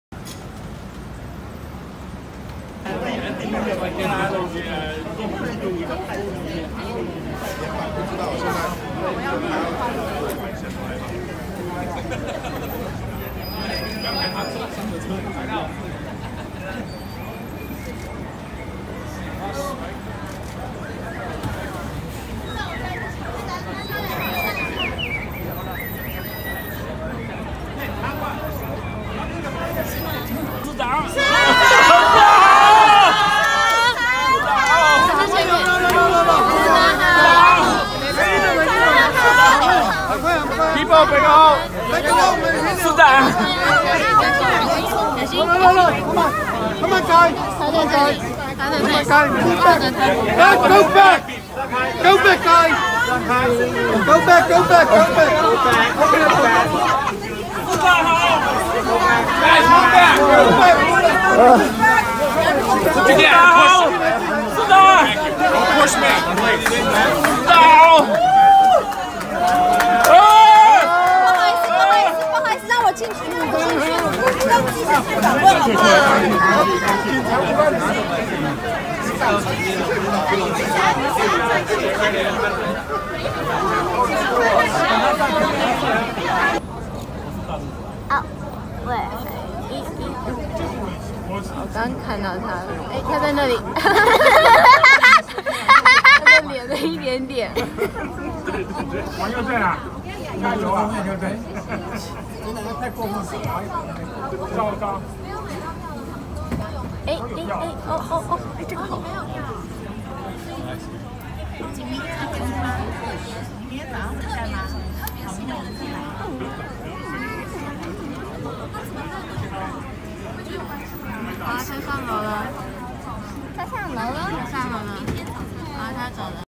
04.11.2019. 韓國瑜波士頓宴會.唐人街, 馬薩諸塞. April 11th, 2019 Han Kuo-Yu dinner banquet in Chinatown, Boston, MA
韓國瑜(Han Kuo-Yu) dinner banquet in Chinatown, Boston, Massachusetts on April 11th, 2019.